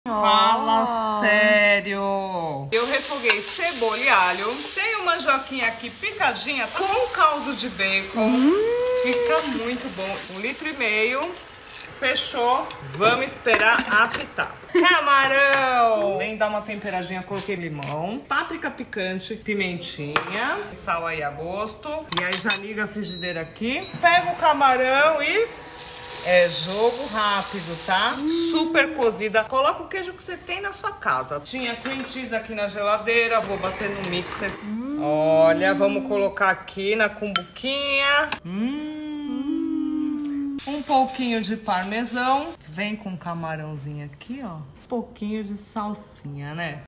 Sopa de mandioquinha com Camarão
Confira a receita completa!

INGREDIENTES:
- 500g de mandioquinha
- 1 cebola
- 2 dentes de alho
- Fio de Azeite
- 1,5l de água
- 2 c. de sopa de creme de ricota
- 2 c. de sopa de cream cheese
- 2 c. de sopa de queijo ralado (muçarela e parmesão)
- 160g de camarão

MODO DE FAZER:
Na panela de pressão, refogue a cebola, o alho e coloque a mandioquinha picada. Cubra com água e deixe cozinhar bem. Assim que cozinhar, junte o queijo, o cream cheese, o creme de ricota e bata tudo no mixer.

Doure o camarão temperado com sal, limão e páprica. Sirva a sopa e finalize com queijo e camarões.